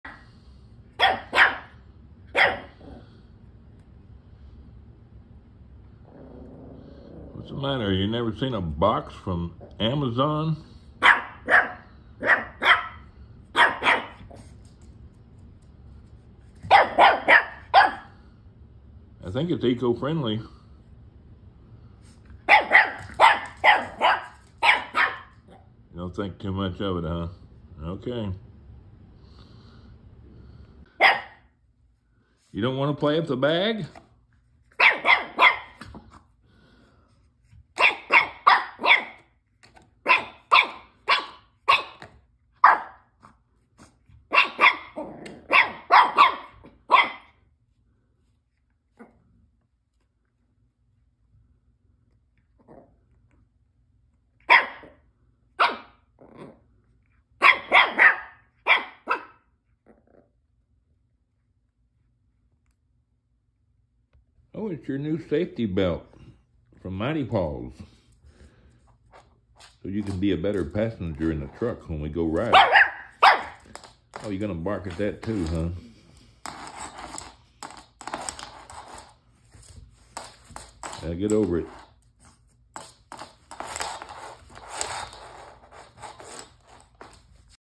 Suzy gets a package in the mail today.
I ordered Suzy a safety belt for my truck.